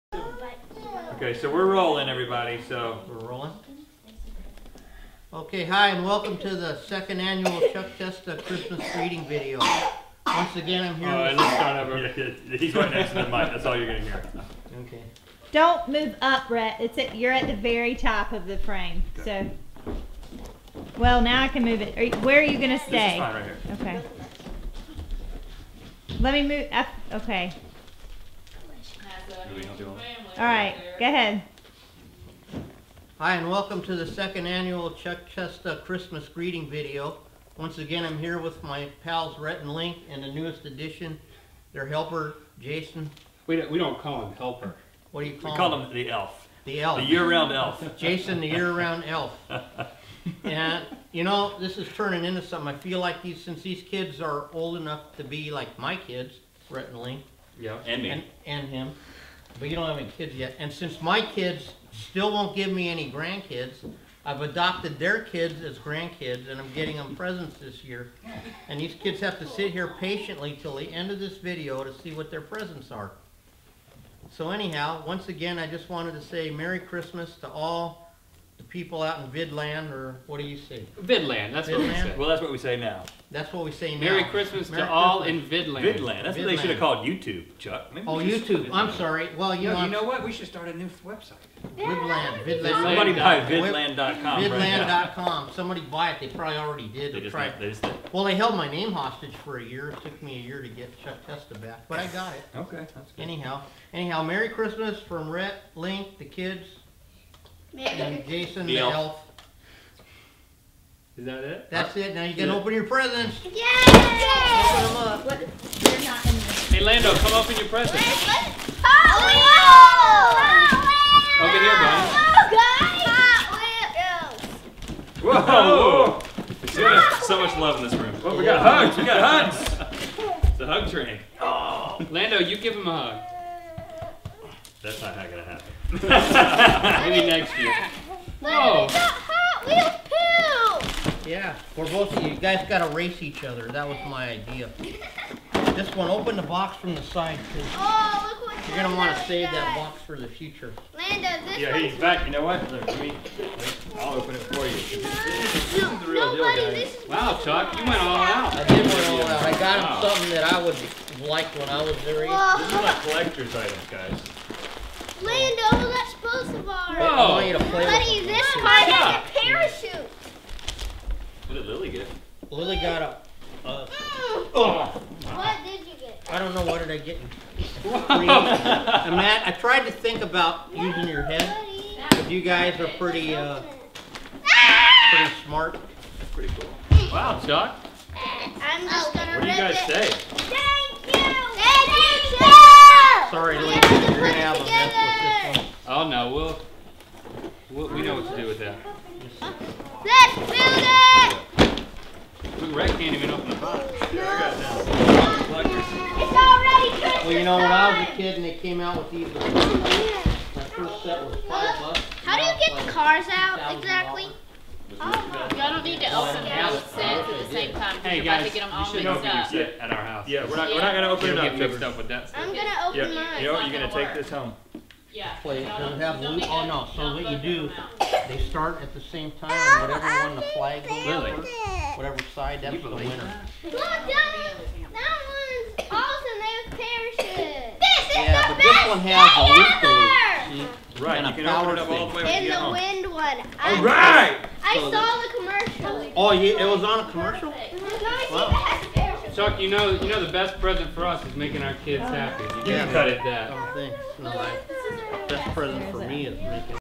Testas Tuesday Tip Christmas With Rhett & Link and Kids !
Chuck, Rhett & Links 2nd Annual Christmas Greeting Video ! Unedited As Always !